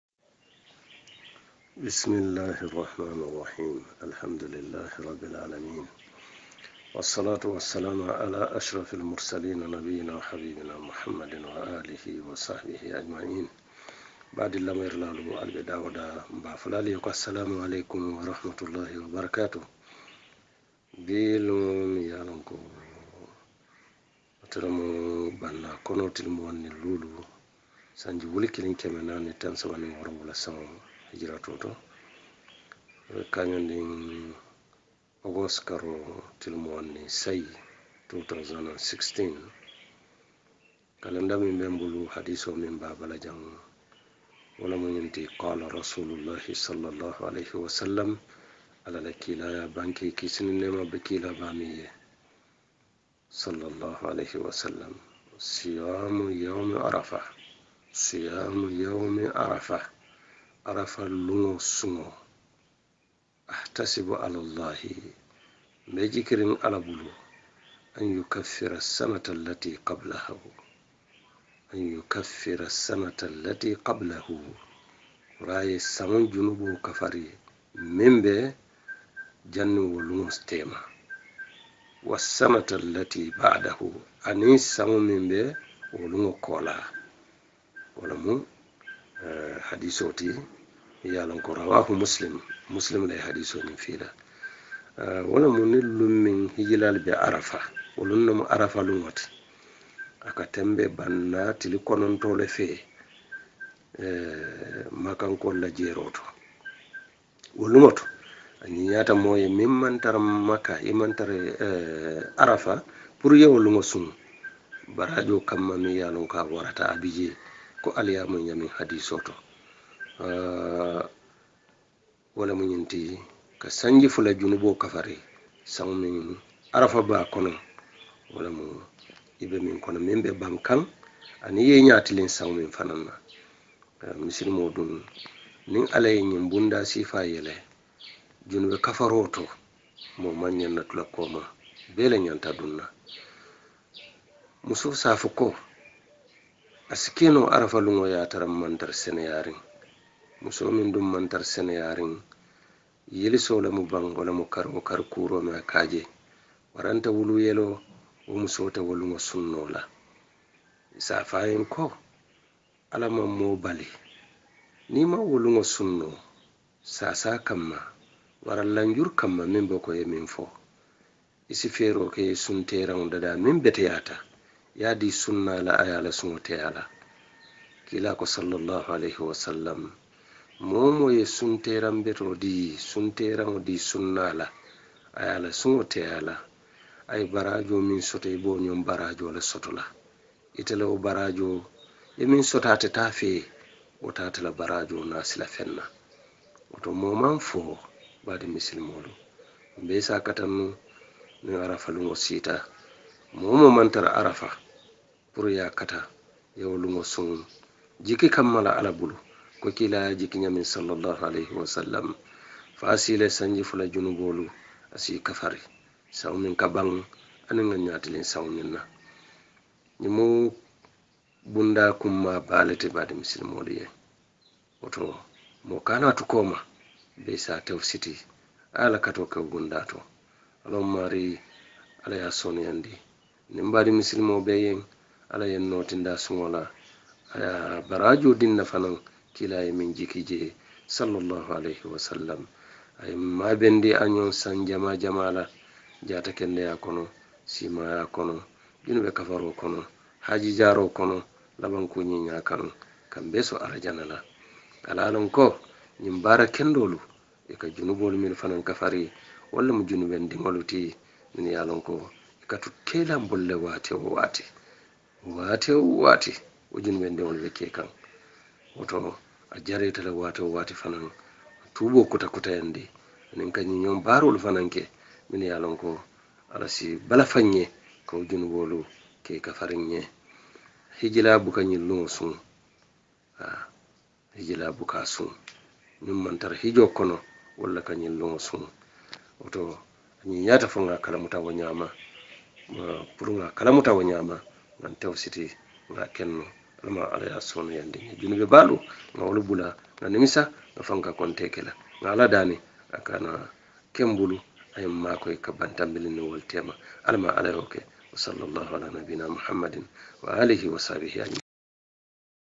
IMAM FATTY (The Significant Of Fasting On The Day Of Arafat.Day 28)
The Significance Of Fasting On The Day Of Arafat.Day 28